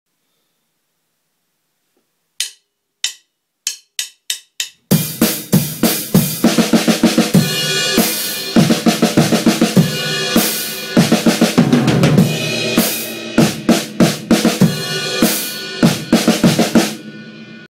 ダンスナンバー 間奏
ダンスナンバーの間奏前フィルから間奏です。